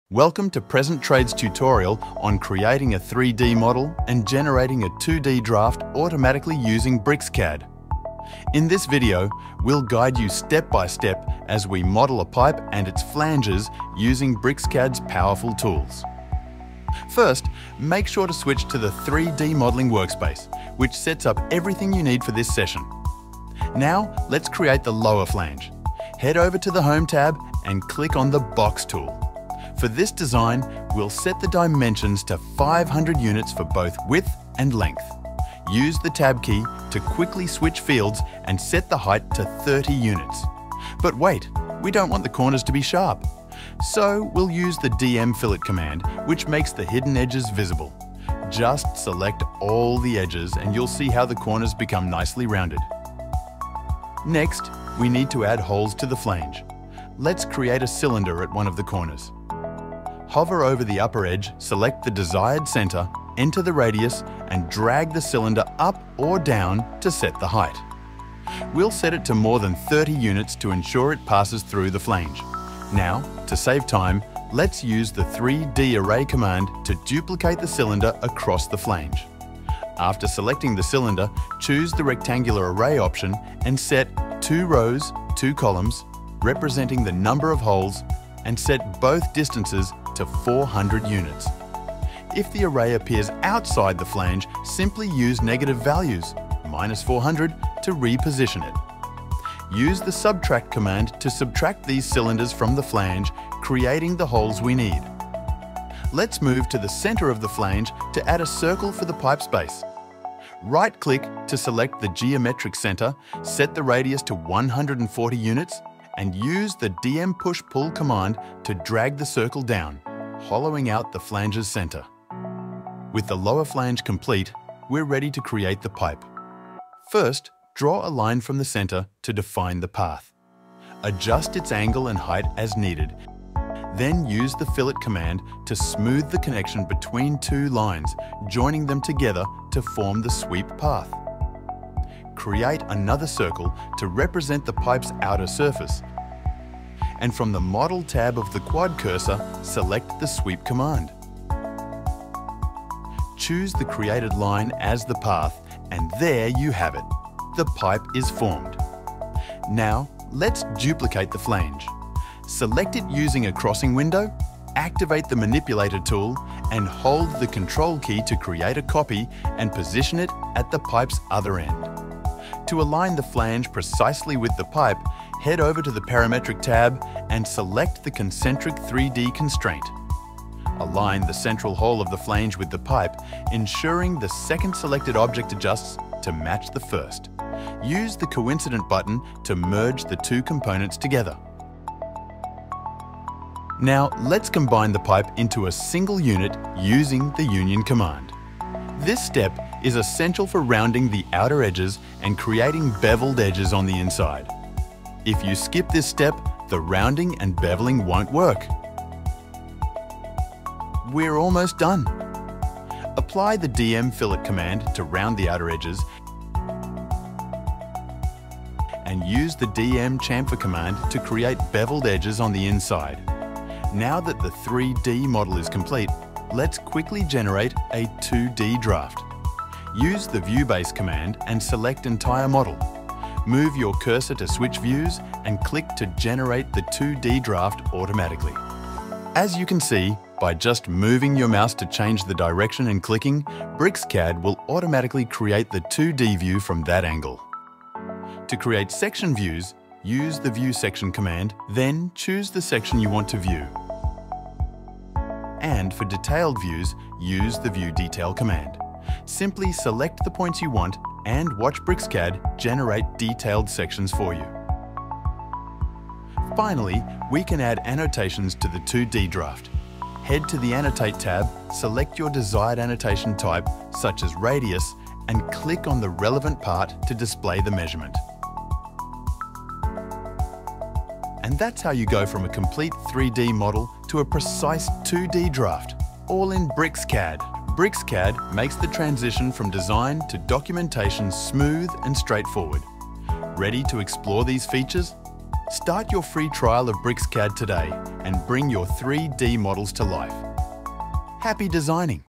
BricsCAD Pro: 002 - How to Create a 3D Model & Automatically Generate a 2D Draft?
Welcome to Present-Trade’s tutorial on how to create a 3D model and automatically generate a 2D draft using BricsCAD Pro.

What You’ll Learn:

Design a 3D model efficiently using BricsCAD’s powerful tools.
Use smart commands to build and modify complex mechanical components.
Generate accurate 2D drafts from your 3D models with a single click.
Explore advanced features for editing and refining drafts automatically.
This step-by-step guide simplifies the process of transitioning from 3D to 2D, ensuring your designs are precise and ready for production.

Whether you’re a mechanical designer, engineer, or CAD enthusiast, this tutorial will help you harness the full potential of BricsCAD Pro.